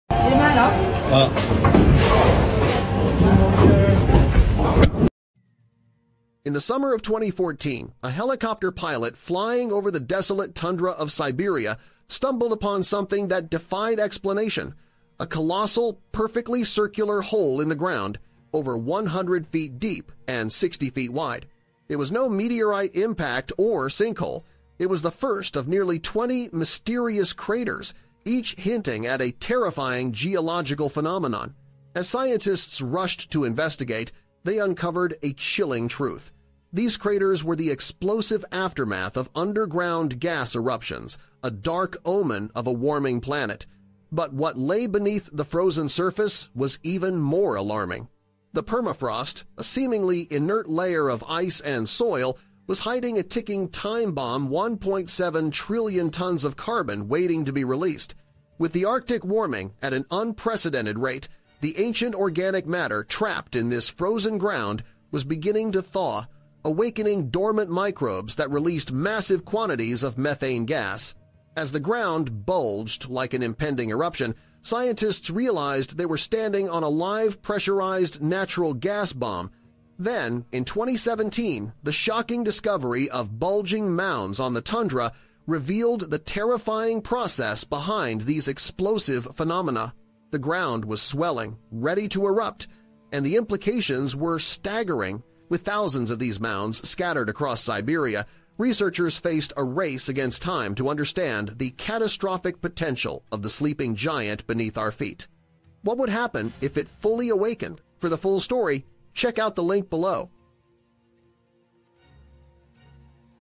Scientists Found Strange Bouncing Hills in Siberia, Then Realized They Were About to Explode
Scientists Found Strange Bouncing Hills in Siberia, Then Realized They Were About to Explode. In the remote, frozen tundra of Siberia, helicopter pilots began discovering massive, perfectly circular craters that seemed to have exploded from the earth. What was causing these eruptions?
This is the incredible and alarming true story of a new geological phenomenon. We delve into the science of the Arctic permafrost, a "sleeping giant" that holds one point seven trillion tons of frozen carbon. As the planet warms, this giant is waking up.
Join us as we follow Russian scientists who, after years of investigation, discover that the craters are just the beginning. They find thousands of bizarre, jelly-like mounds bulging from the ground, which are actually high-pressure pockets of explosive methane gas. This is the story of a natural minefield of seven thousand ticking time bombs and a terrifying warning from the front lines of climate change.